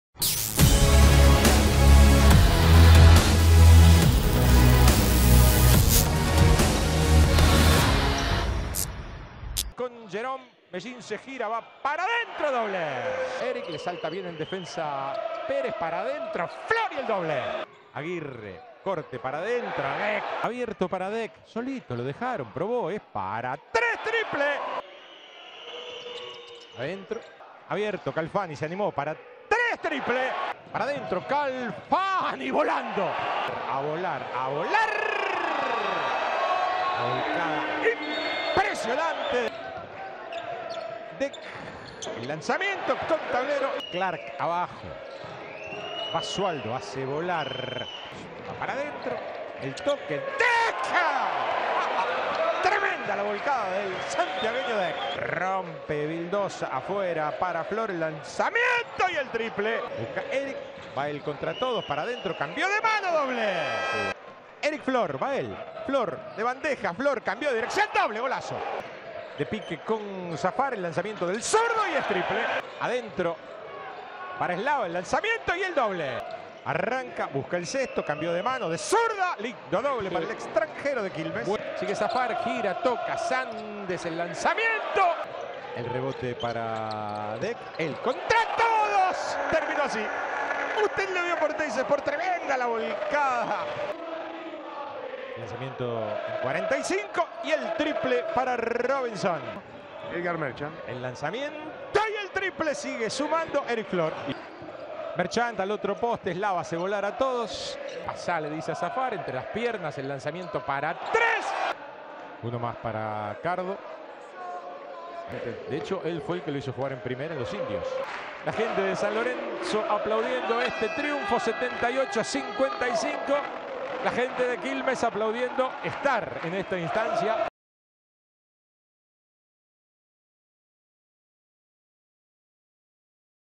21.06.2017 | San Lorenzo vs. Quilmes
Triunfo de San Lorenzo (78-55) sobre Quilmes de Mar del Plata, en el Polideportivo Roberto Pando de Capital Federal, para ubicarse 2-0 en la serie final de Conferencia Sur.